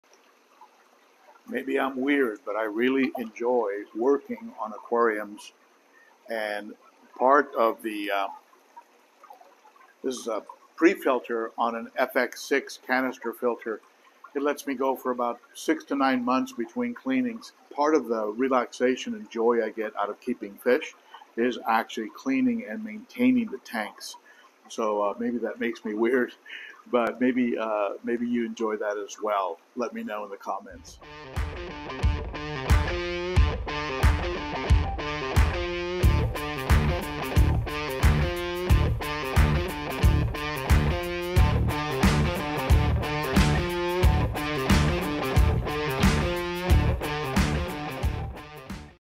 How Does this *Weirdo* Relax?
I find working on my aquariums very relaxing and rewarding. Do you?

Join a helpful Facebook group here: Ben O'Cichlid